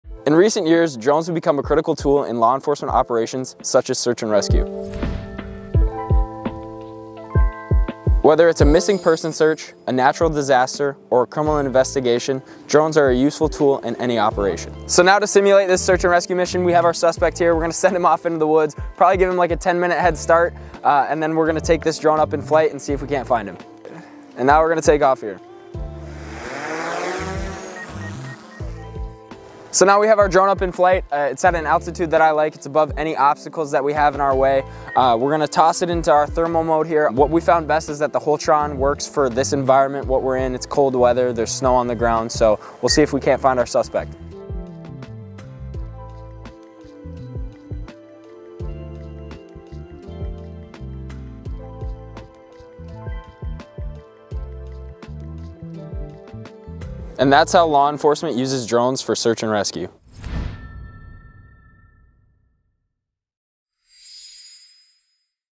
Drones for Search & Rescue Using Thermal | DJI Mavic 3T | Maverick Drone Systems
In this video, we will explore how law enforcement agencies are using drones with thermal imaging technology for search and rescue operations. Drones equipped with thermal imaging cameras have become increasingly popular tools for rescue teams, as they provide a bird's eye view of difficult-to-reach areas and can quickly locate people in distress.

What we used to film!
Canon R6
Canon RF 24-70mm 2.8
DJI Mics

Checkout out our other social media accounts!